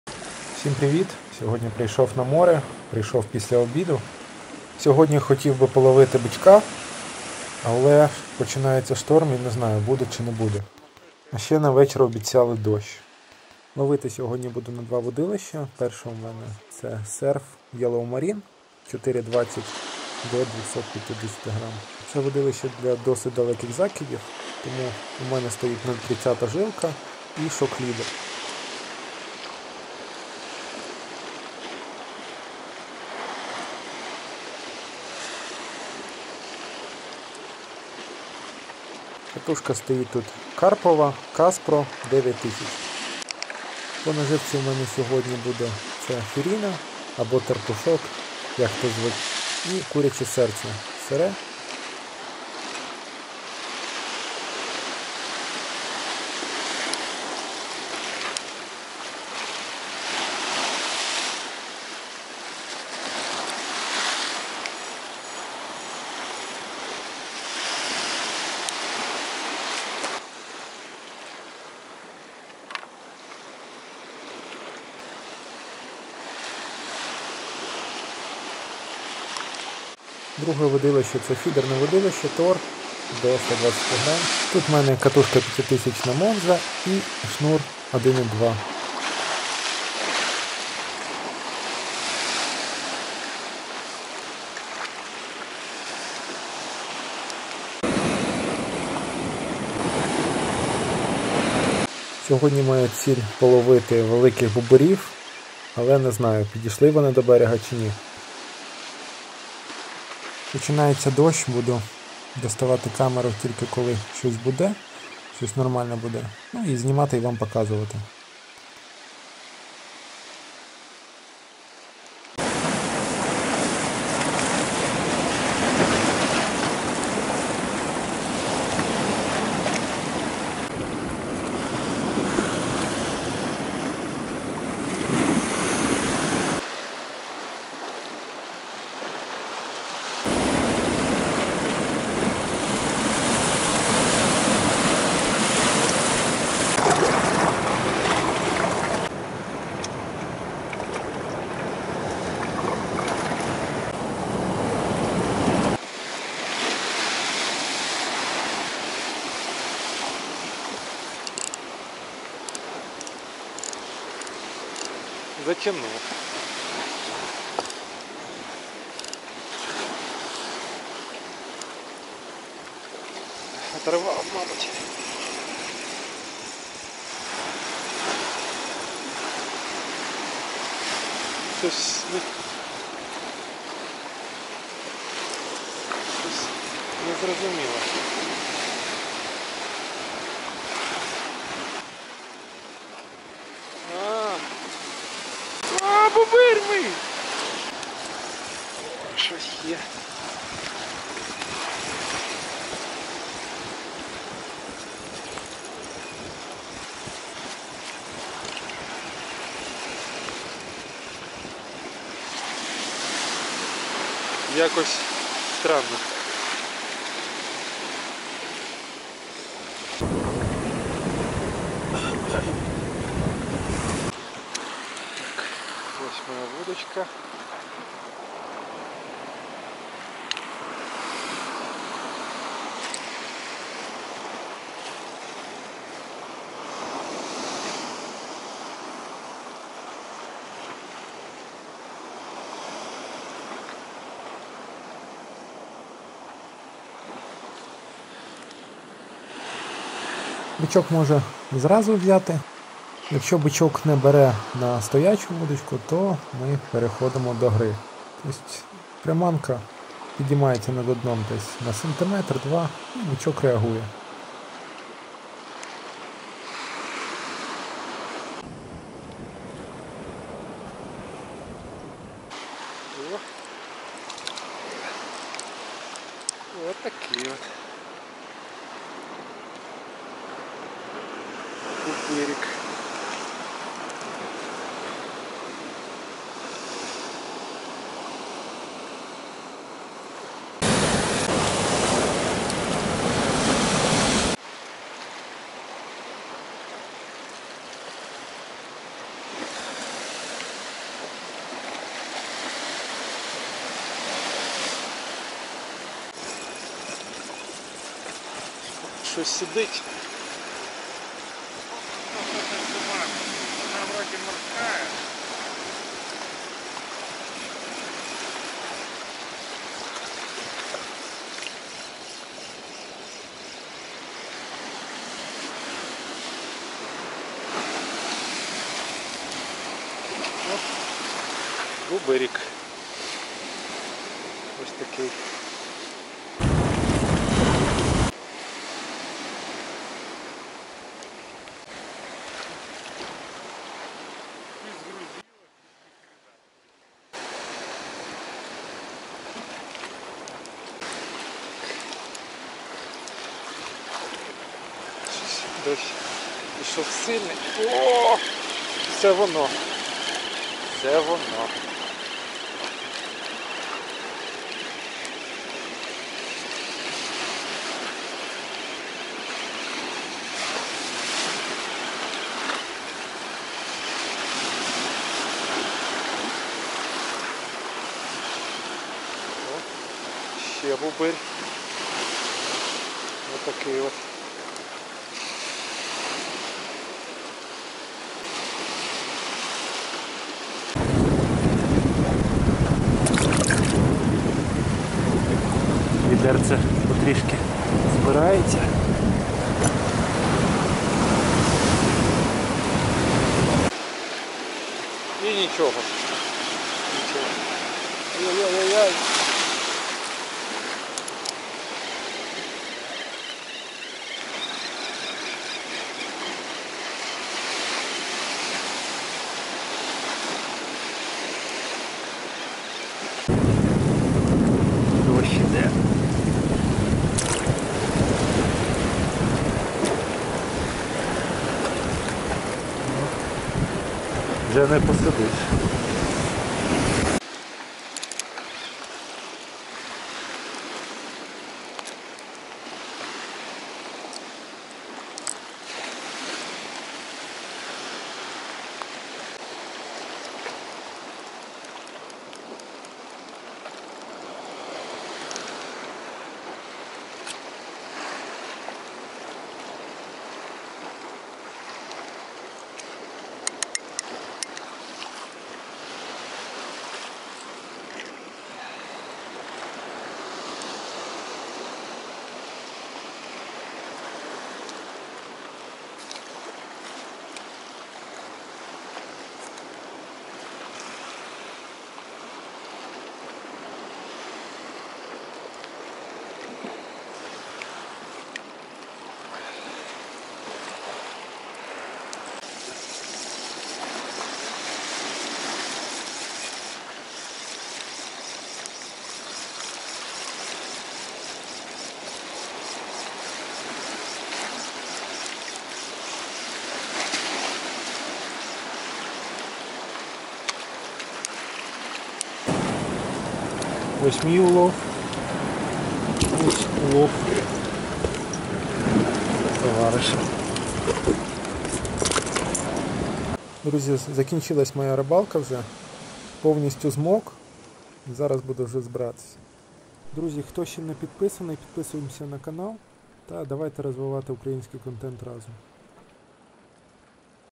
Ловимо Бичка в дощ🌧️🌧️.ЧОРНЕ МОРЕ радує.Рибалка з пірсу в шторм🇺🇦
снасті які я використовував в цьому відео:
Вудилище Yellow Marine Surf LC 4.20m 100-250g